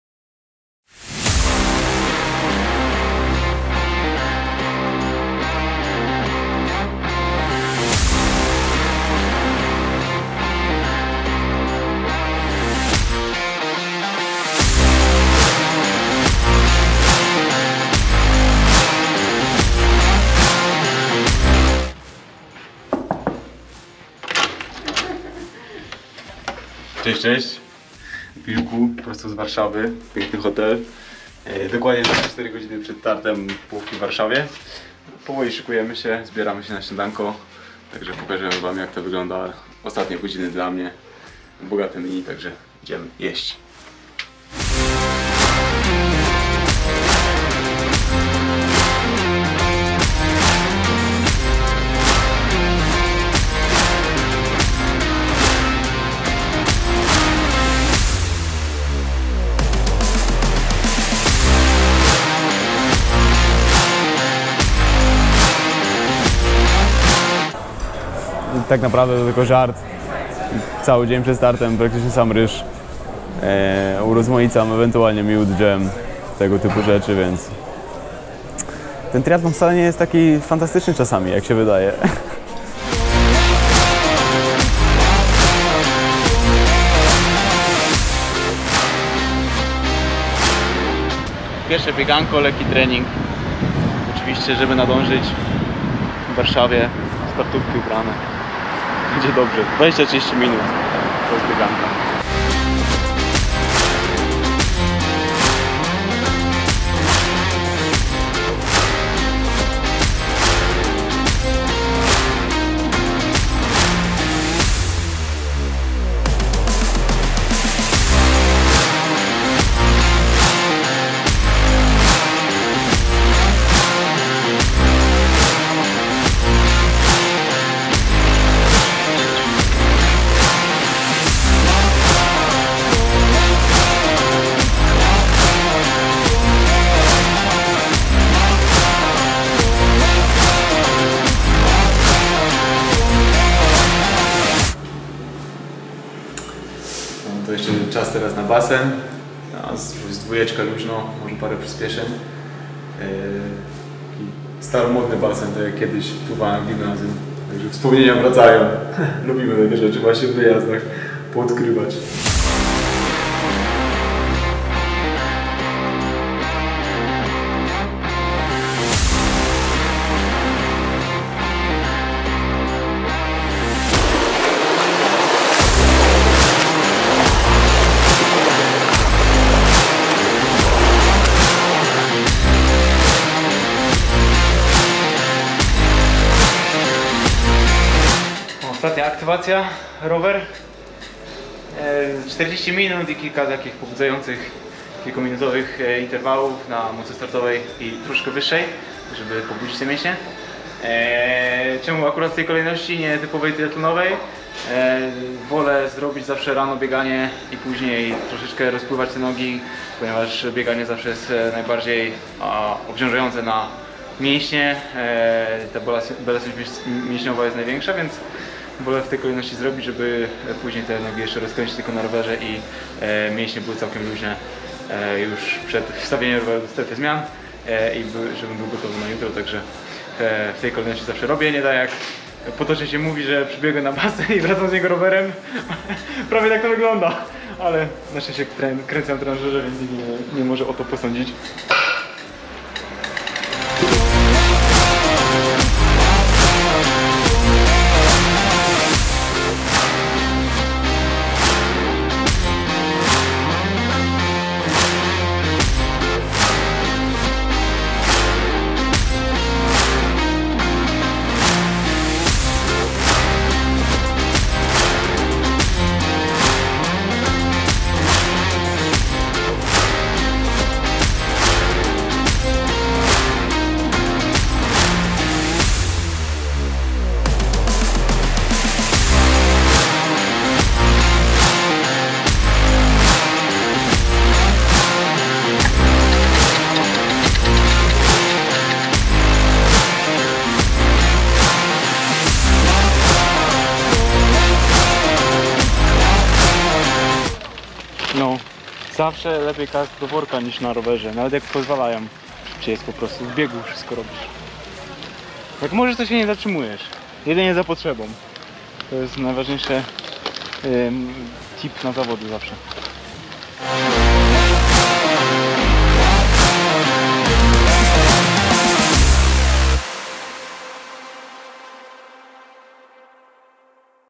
[ENG Sub] IRONMAN 70.3 Warsaw | A DAY BEFORE THE RACE
Jak wyglądają moje 24h przed wyścigiem na połówce IRONMAN:
- Jak trenuję na 24h przed zawodami IRONMAN 70.3 ?
- Co jem przed zawodami triathlonowymi?
- Dlaczego treningi przed wyścigiem IRONMAN wykonuję w takiej kolejności?
- Obowiązki PRO zawodnika triathlonu;)

Jak wy spędzacie czas przed swoimi startami ?

Film by: @Photos_How